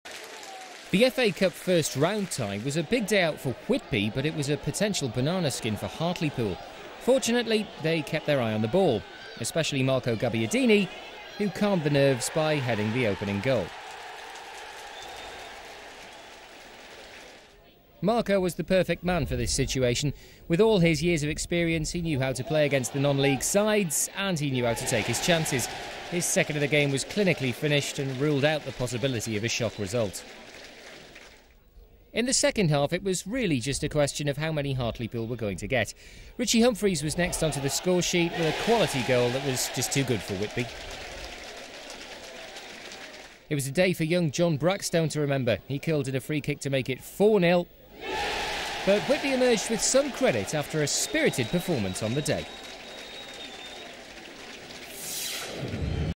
Hartlepool United 4-0 Whitby Town - 8th November 2003 (FA Cup R1)
(1-0) Marco Gabbiadini 25'
(2-0) Marco Gabbiadini 30'
(3-0) Ritchie Humphreys 51'
(4-0) John Brackstone 68'

HUFC: Jim Provett, Micky Barron (Darren Craddock 73), Michael Nelson, Chris Westwood, John Brackstone, Gavin Strachan, Mark Tinkler, Ritchie Humphreys, Eifion Williams, Paul Robinson (Jack Wilkinson 54), Marco Gabbiadini (Darrell Clarke 76) // Manager - Neale Cooper

WTFC: Adam Clementson, Adam Reed (Mark Swales 86), Tony Hall, Mark Obern (Lee Ure 57), Brian Linighan, Graham Robinson, Graham Williams, Craig Veart, Anthony Ormerod, Ben Dixon, Gary Browne // Manager - Harry Dunn